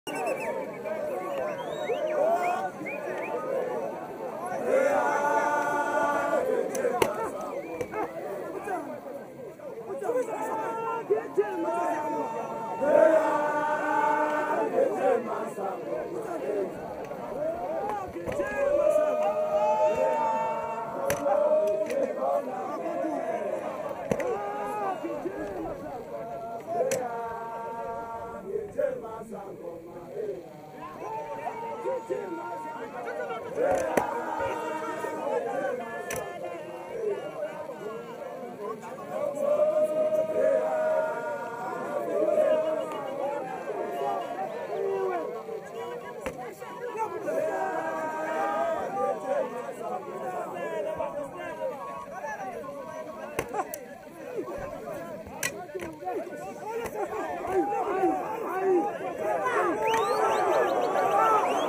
SANGOMA VS JUKEBOX
TWO GREAT WARRIORS TEST EACH OTHERS SKILLS